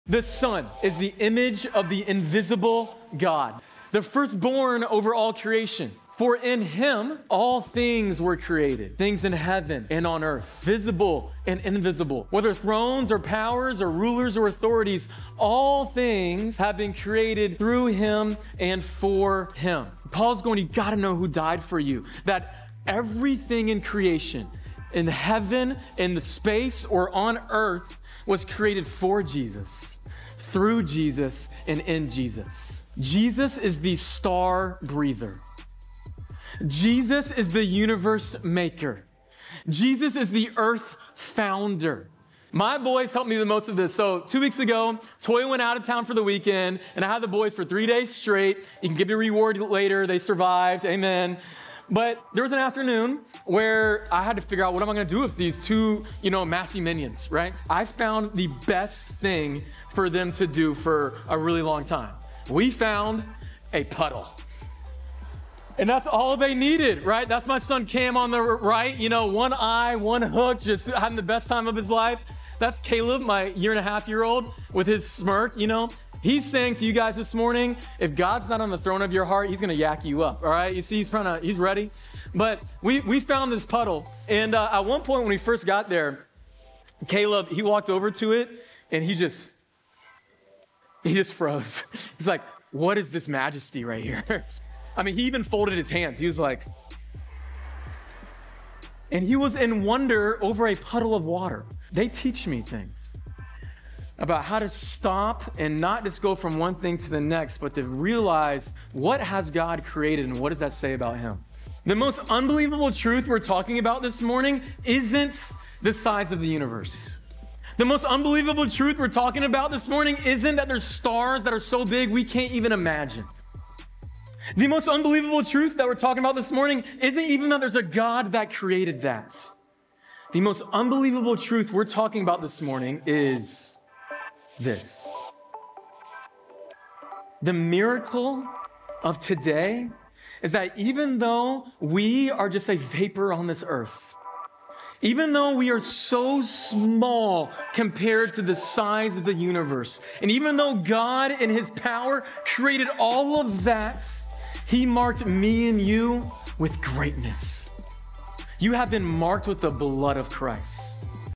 The Greatness of the Universe - Col. 1:15-16 | Jordan Massey
"It's about how to stop and not just go from one thing to the next, but to realize what has God created."

Jordan this past Sunday for Easter broke down the vastness of the universe and how small we are compared to what God has created! Happy post-Easter, hope that everyone has an awesome Friday!!

God bless!

Colossians 1:15-16

"The Son is the image of the invisible God, the firstborn over all creation.
For in him all things were created: things in heaven and on earth, visible and invisible, whether thrones or powers or rulers or authorities; all things have been created through him and for him."